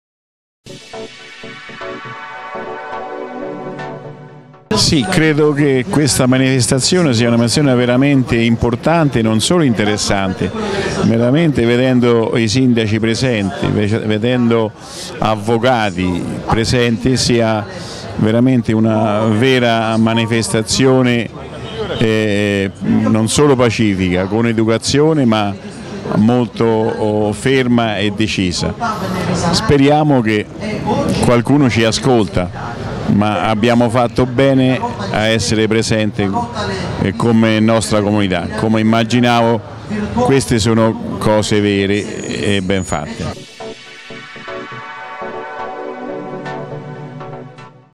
Manifestazione ANCI contro la Spending rewiev, il commento di Evasio Gialletti
Martedì 24 luglio 2012 l'Anci nazionale ha indetto una manifestazione per protestare contro la spendig rewiv del governo Monti, che rischia di tagliare utleriormente le risorse dei Comuni italiani. Anche da Orvieto è partito un pulman con le autorità cittadine e un folto gruppo di avvocati per cercare di impedire la chiusura del tribunale di Orvieto.